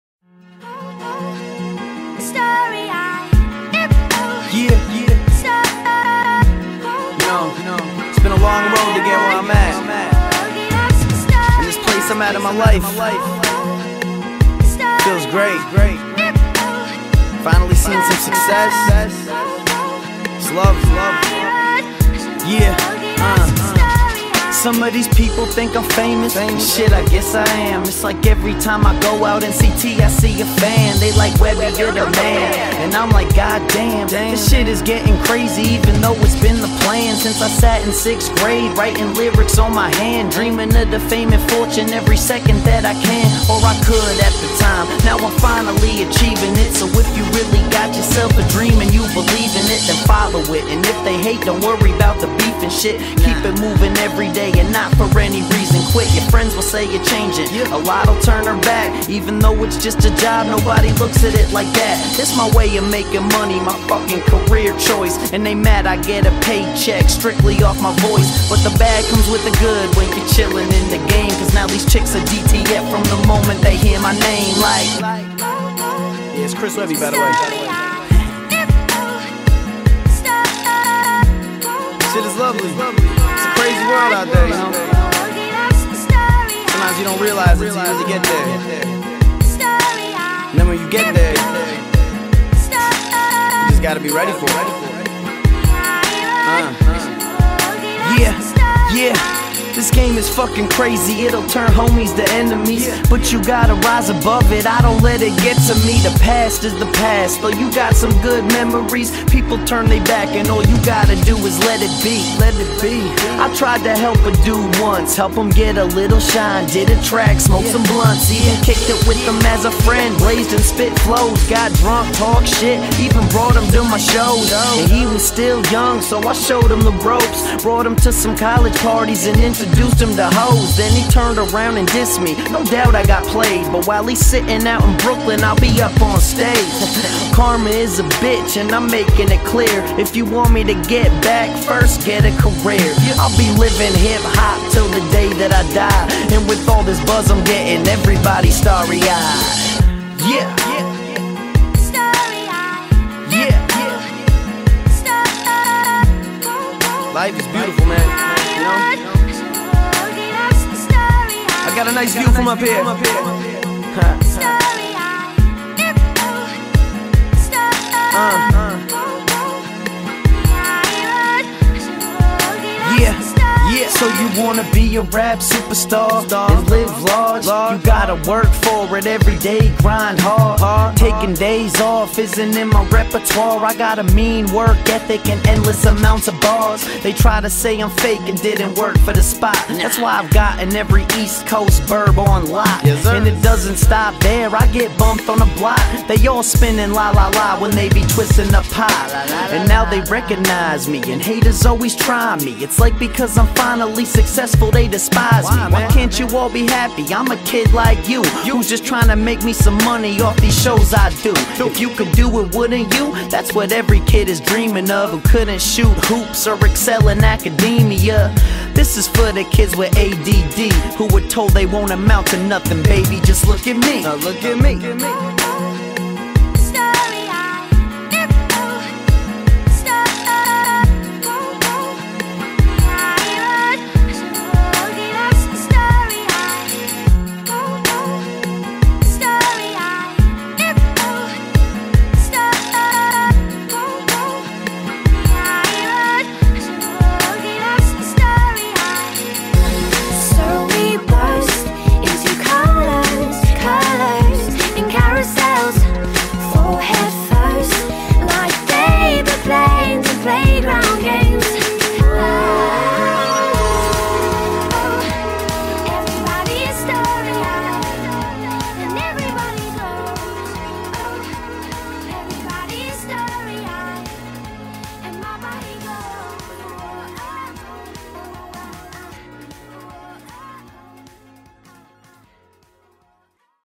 Graffiti Piece #2 (Philly 215)
Mural done in Philadelphia on the back of a community center